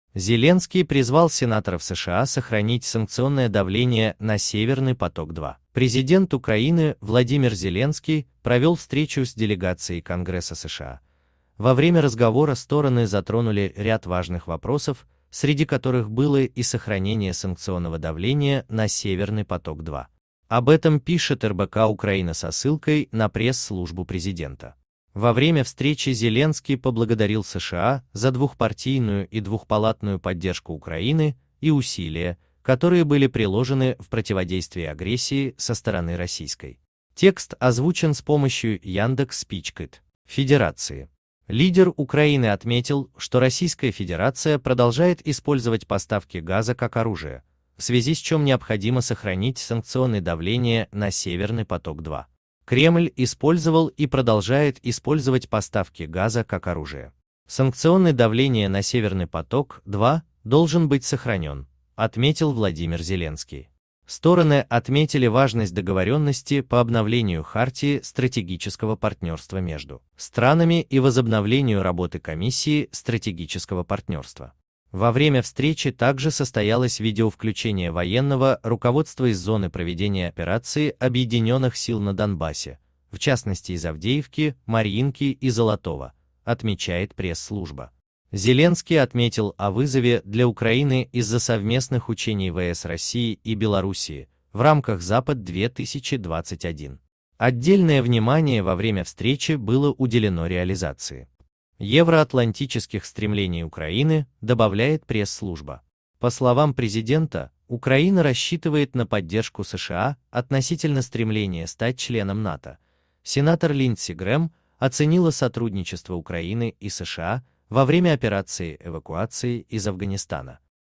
Зеленский призвал сенаторов США сохранить санкционное давление на "Северный поток-2".
ВладимирЗеленский СоединенныеШтатыАмерики СенатСША Северныйпоток-2 Санкции Президент Президент Украины Владимир Зеленский провел встречу с делегацией Конгресса США. Во время разговора стороны затронули ряд важных вопросов, среди которых было и сохранение санкционного давления на "Северный поток-2". Об этом пишет РБК-Украина со ссылкой на пресс-службу президента. Во время встречи Зеленский поблагодарил США за двухпартийную и двухпалатную поддержку Украины и усилия, которые были приложены в противодействии агрессии со стороны Российской Федерации. Лидер Украины отметил, что Российска